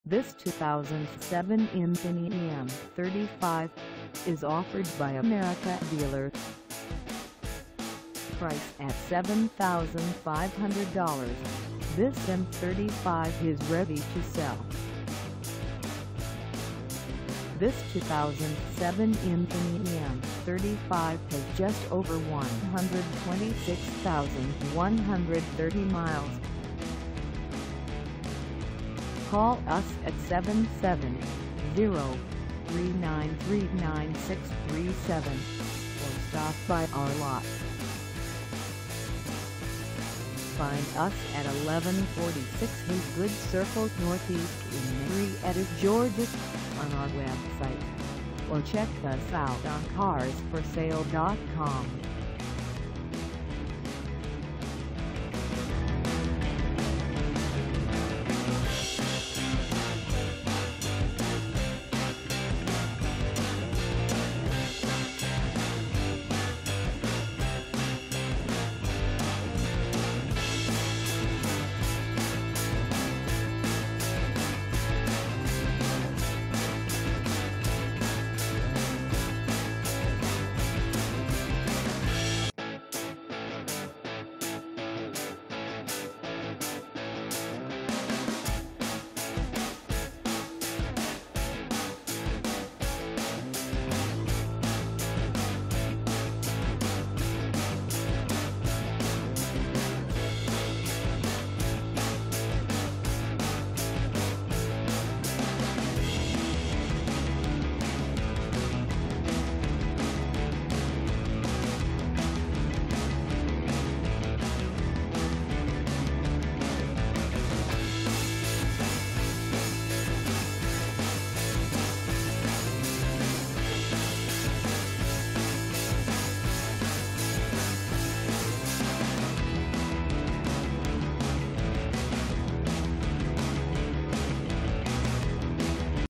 2007 Infiniti M35 x AWD 4dr Sedan for sale in MARIETTA, GA 3
This 2007 Infiniti M35 x AWD 4dr Sedan is for sale in MARIETTA, GA 30062 at AMERICA DEALER.

Engine: 3.5L V6
Transmission: Automatic 5-Speed
Color: Gray
Mileage: 126130
Contact AMERICA DEALER 1146 HAGOOD CIRCLE  NE  MARIETTA, GA 30062 to test drive this 2007 Infiniti M35 x AWD 4dr Sedan today.